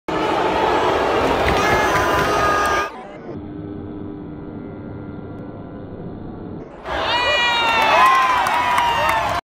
Ben Bishop shootout save on Peter Holland 3/14/17
Kings vs Coyotes shootout 3/14/17